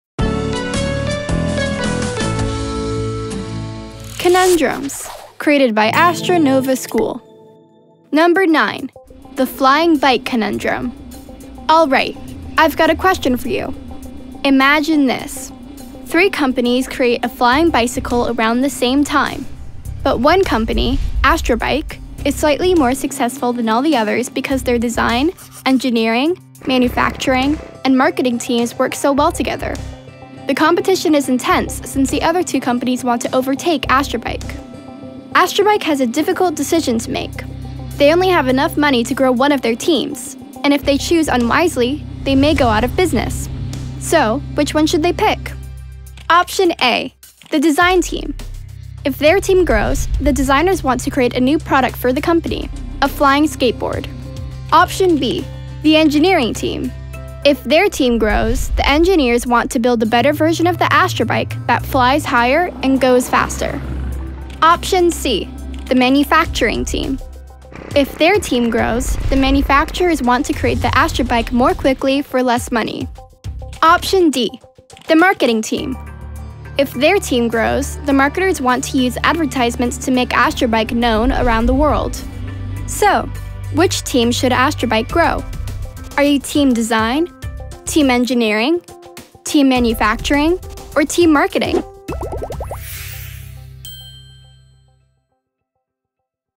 #9 The Bike Conundrum
Number Nine: The Flying Bike Conundrum

Alright, I’ve got a question for you. Imagine this. Three companies create a flying bicycle around the same time, but one company, AstraBike, is slightly more successful than all the others because their Design, Engineering, Manufacturing, and Marketing teams work so well together. The competition is intense since the other two companies want to overtake AstraBike.

AstraBike has a difficult decision to make. They only have enough money to grow one of their teams, and if they choose unwisely, they may go out of business. So, which one should they pick?

Option A: The Design Team. If their team grows, the designers want to create a new product for the company...a flying skateboard.

Option B. The Engineering Team. If their team grows, the engineers want to build a better version of the Astra Bike that flies higher and goes faster.

Option C: The Manufacturing Team. If their team grows, the manufacturers want to create the AstraBike more quickly for less money.

Option D The Marketing Team. If their team grows, the marketers want to use advertisements to make AstraBike known around the world.

So, which team should AstraBike grow? Are you Team Design, Team Engineering, Team Manufacturing, or Team Marketing?

Writing: Julian Dans, Josh Dahn, and the Astra Nova Conundrums Team
Animation: Igor Coric, ARTRAKE
Voice: Nova, Carasmatic Productions
SFX and Music: Matthew Pablo
Sound Mixing: David Dans, Carasmatic Productions